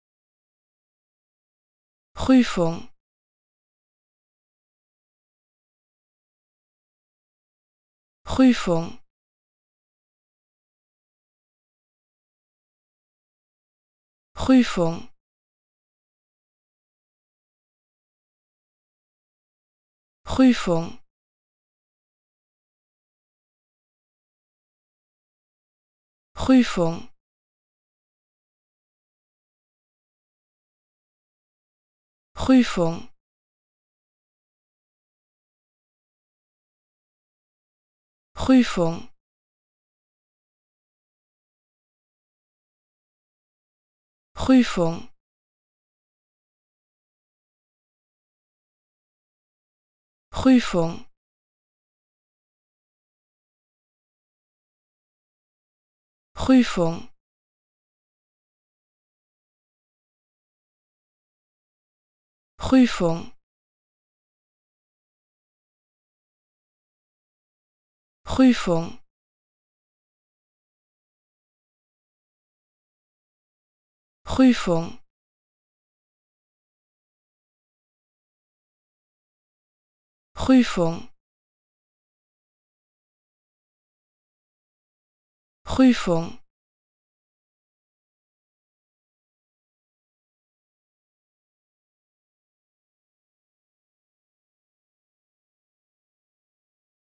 How to Pronounce ''Prüfung'' (Examination) Correctly in German
Learn how to say ''Prüfung'' in German with this short pronunciation tutorial.